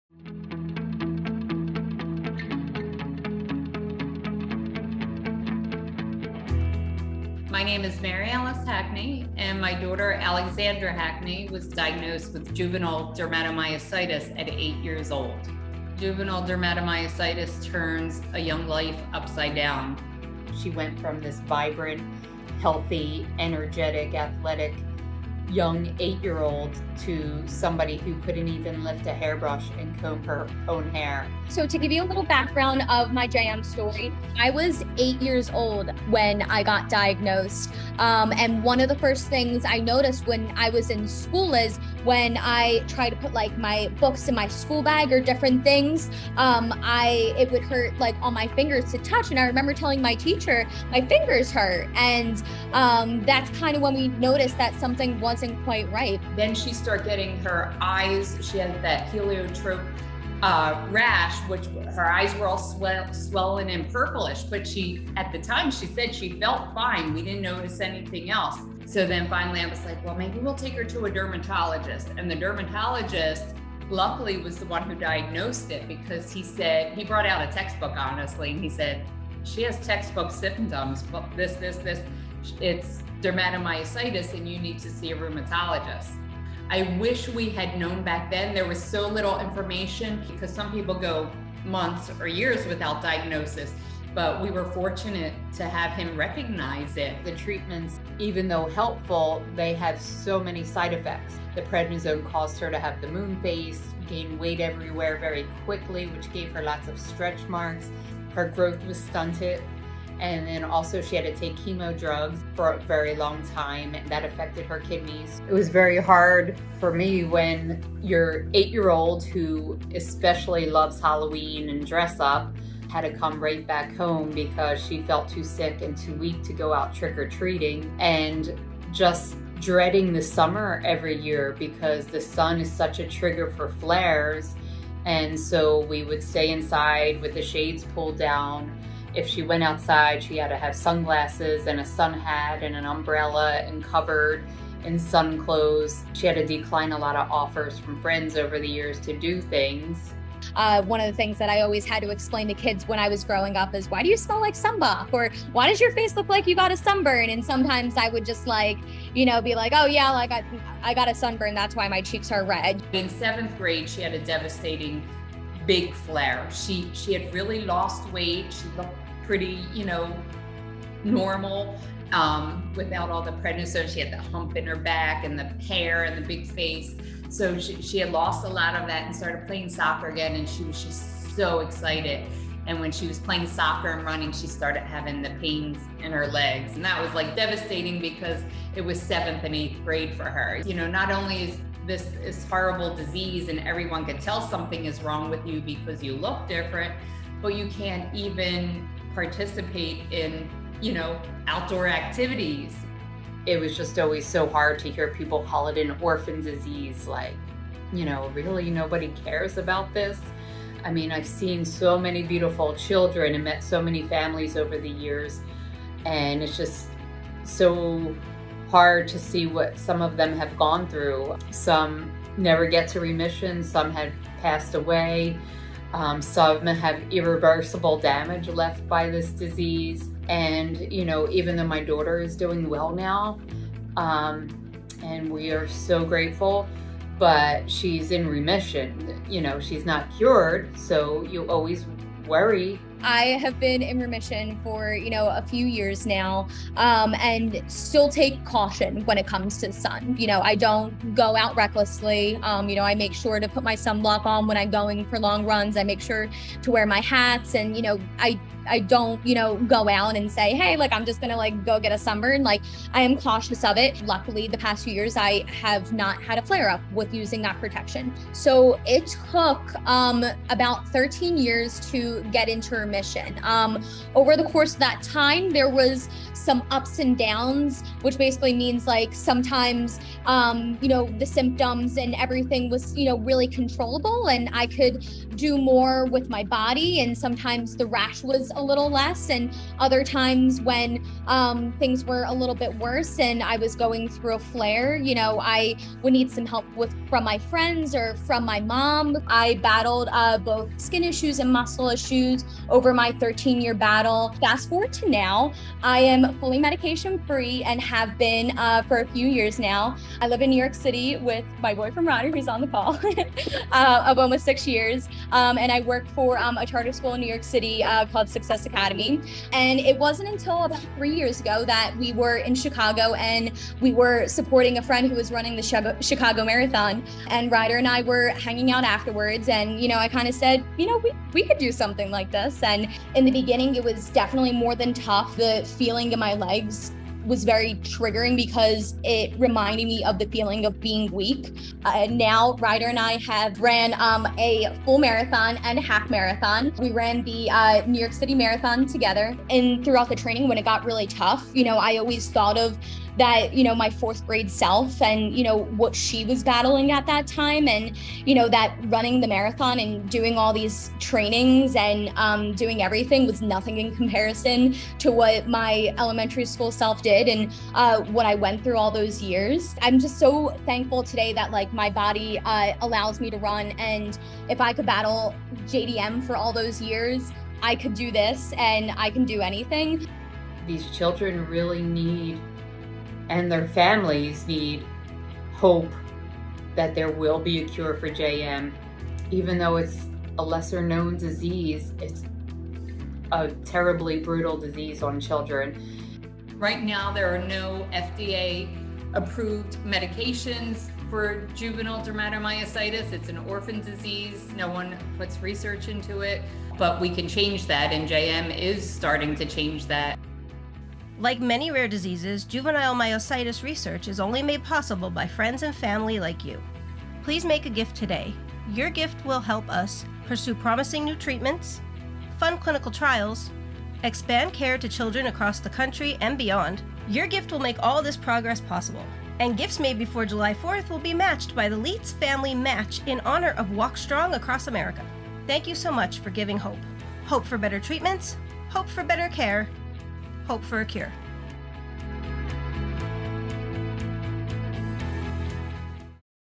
Alexandra’s Story of Hope
Alexandra grew up in Philadelphia. At 8 years old, she was diagnosed with juvenile dermatomyositis. Her first symptom was pain in her cuticles whenever she would touch something. Then came the rash, weakness, painful inflammation, and a host of other symptoms. Her strength and mobility were heavily impacted. She couldn’t bend over to pick things up. She couldn’t lift a brush to her hair. She couldn’t even sit on the floor with her legs crossed. Her speech was hard for some to understand due to inflammation and weakness in her throat muscles. Daily activities were painful, and her mom would regularly wash her hair for her. But during her over 13 year long battle with JDM, the rash would be her main symptom.

Alex’s treatment was long-winded. She took Prednisone from ages 8 to 20, regularly receiving oral steroids and IVIG and IV steroids. She didn’t stop IV treatment until she was in college, and a port was placed in her chest because her veins were giving out from the years of needles.

JDM is isolating for children. It doesn’t make you feel like a normal kid. To be known and loved in the midst of the struggle is what gives hope and dignity to those fighting JDM. Alex was lucky to find that in her best friend Caitlin. Since they were little, Caitlin has stood by her side with support and care.

“She always stuck up for me in elementary school during the tough years when kids weren’t so nice.”

Alex credits the combination of all these people as the source of her hope and encouragement, and their community was essential to her developing a fighting spirit and competitive edge.

Thankfully, Alex has been fully medication free since she was 23 years old. Since then, she’s had virtually no symptoms. During the journey and in the long overdue period of relief, her ambition and strength have carried her far. She studied psychology and biology for her undergraduate years before attending graduate school, and she now works as a school psychologist, recently winning an Excellence Award from her workplace.

She ran her first marathon in November and is currently training together with her boyfriend Ryder to run another.

Her offering of hope is heartfelt, and it comes from the place of someone who knows firsthand how difficult this journey can be. Gratefulness for others and courage to see the good are tools Alex has harnessed. She now uses them to enjoy the life before her.